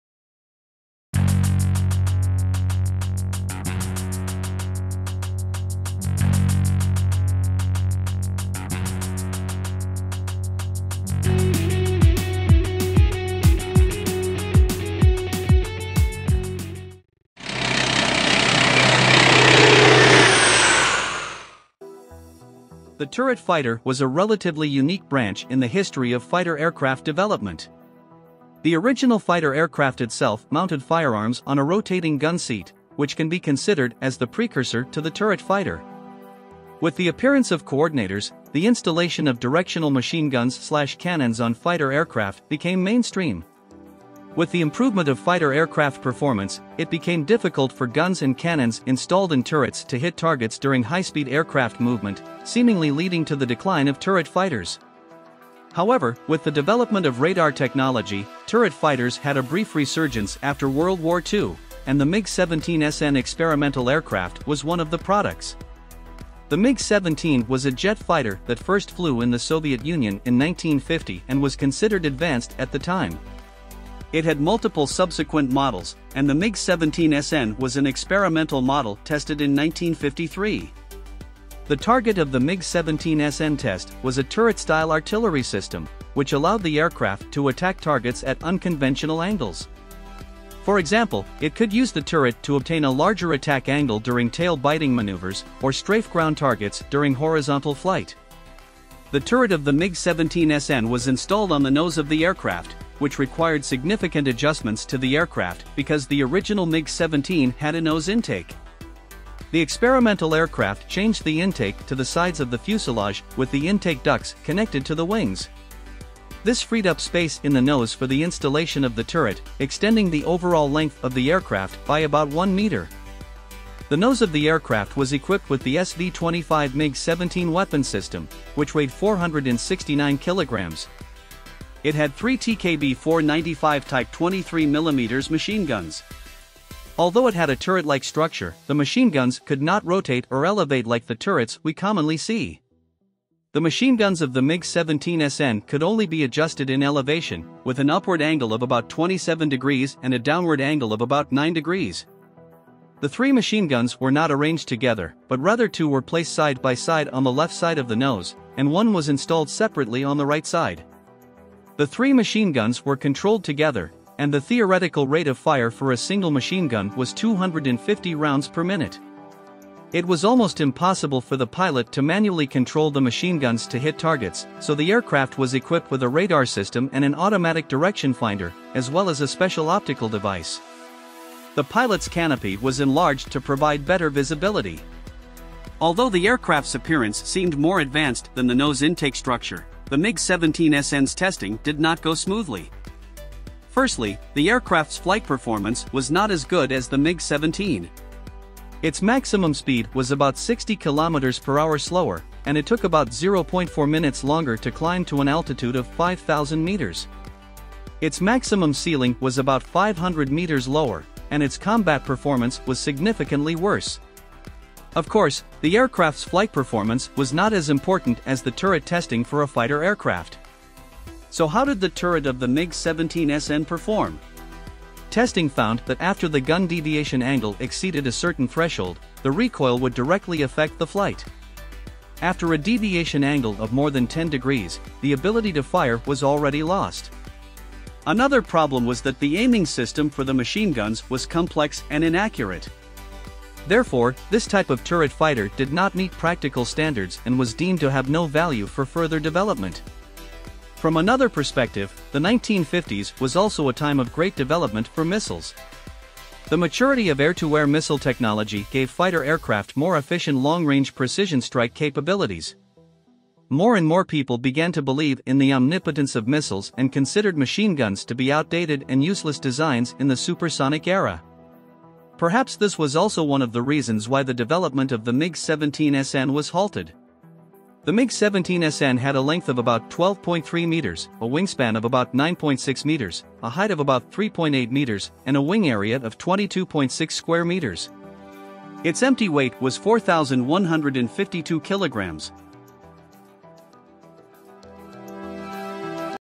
"Revolutionary Turret Fighter: The MiG-17SN and its Unconventional Arsenal"
The MiG-17SN was an experimental jet fighter developed by the Soviet Union in the 1950s. It featured a turret-style artillery system installed on the nose of the aircraft, but due to various technical issues and the rise of missile technology, the development of the MiG-17SN was ultimately discontinued.